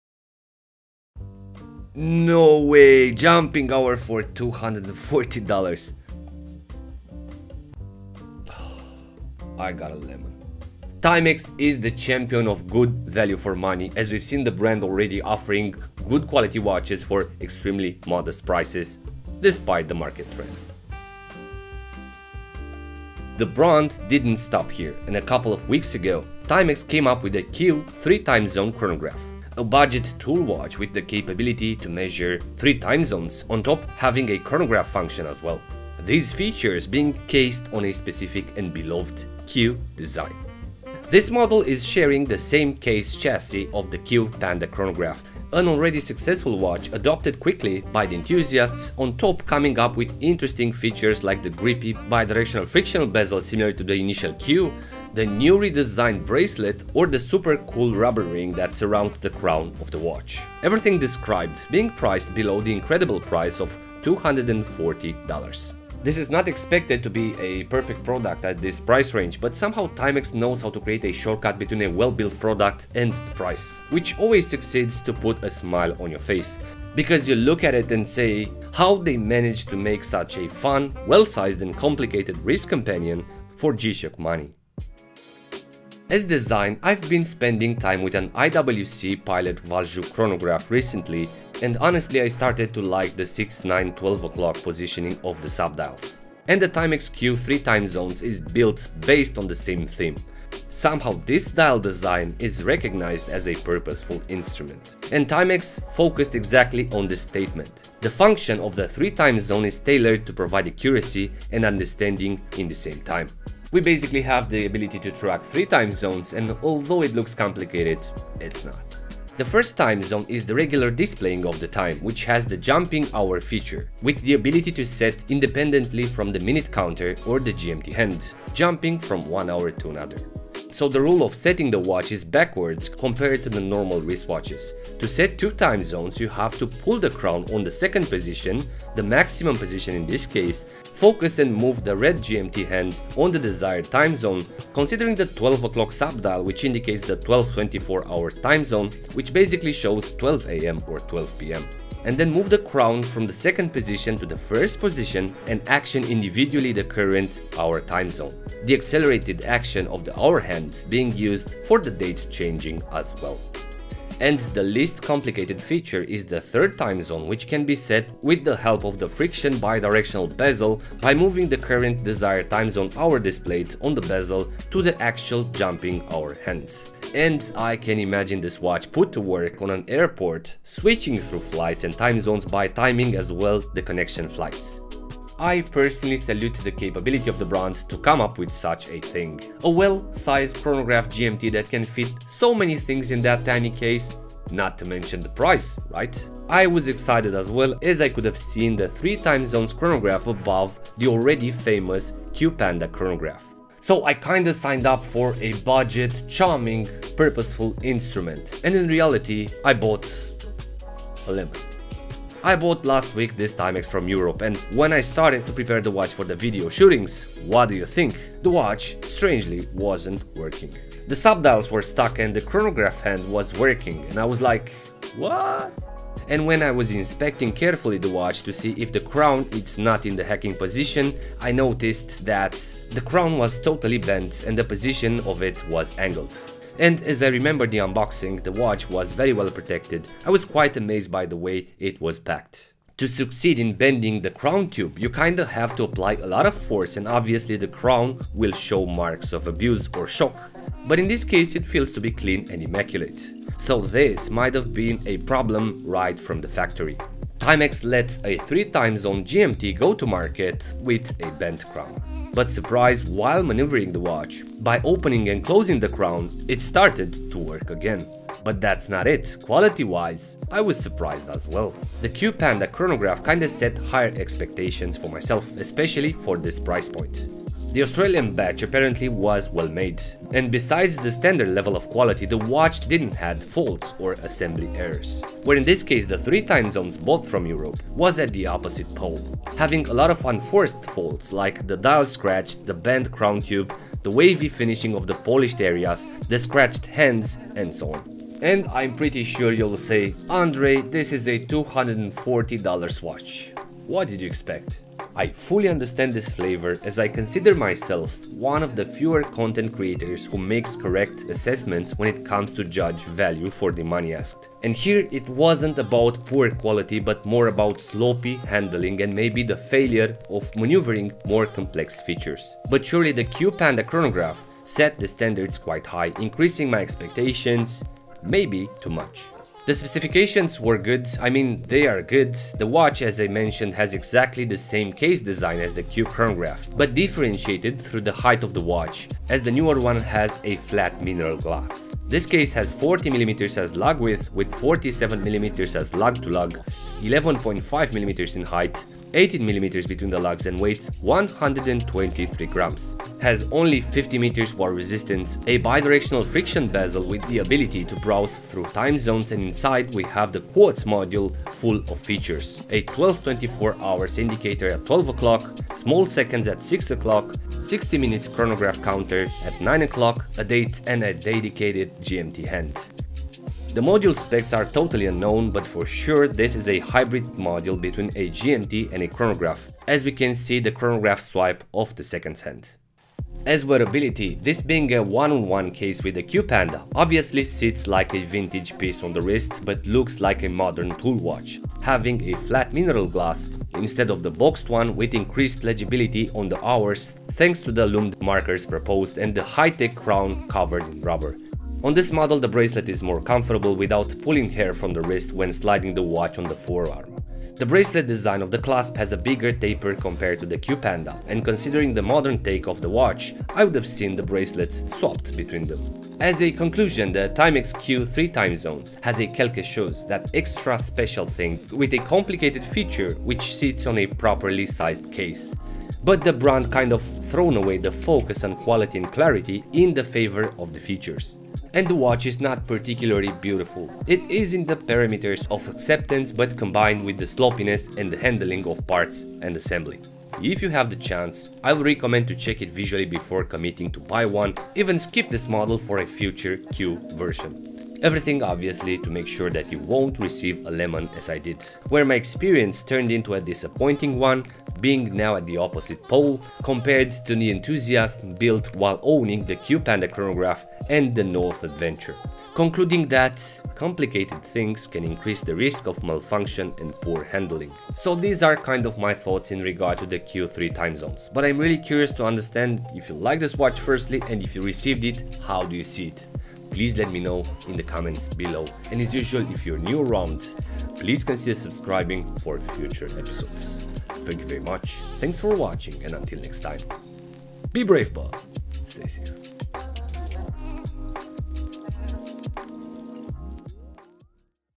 You can easily SKIP the new Q Timex 3 Time Zone Chronograph
Timex Q again! The hype is real, the love is real, the quality is not real this time. As we can see preview in the video, Timex changed a bit the politics of quality for this Q Timex 3 Time Zone Chronograph TW2V69800VQ.

Andrei